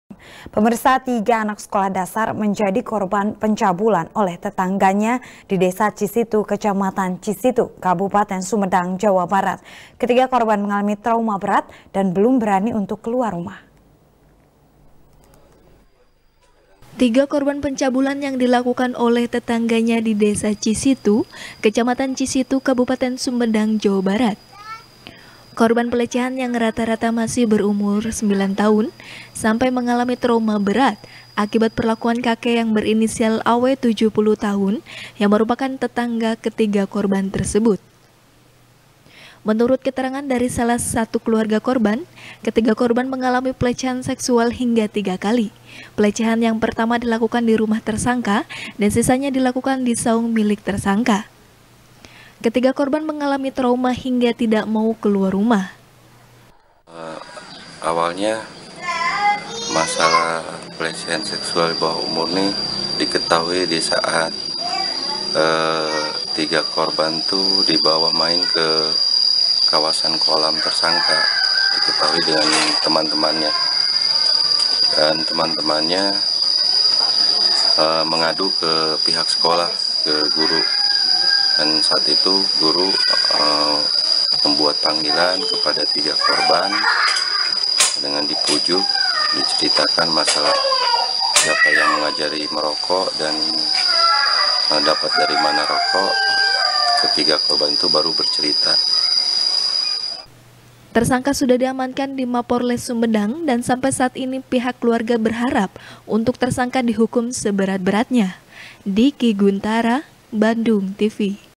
Tiga Anak Sd Menjadi Korban Pelecehan Dari Tetangganya | SBR | BANDUNGTV
SBR | BANDUNGTV | NIKEN PRATIWI | Tiga anak sekolah dasar menjadi korban pencabulan oleh tetangganya, di Desa Cisitu, Kecamatan Cisitu, Kabupaten Sumedang, Jawa Barat . Ketiga korban mengalami trauma berat dan belum berani untuk keluar rumah.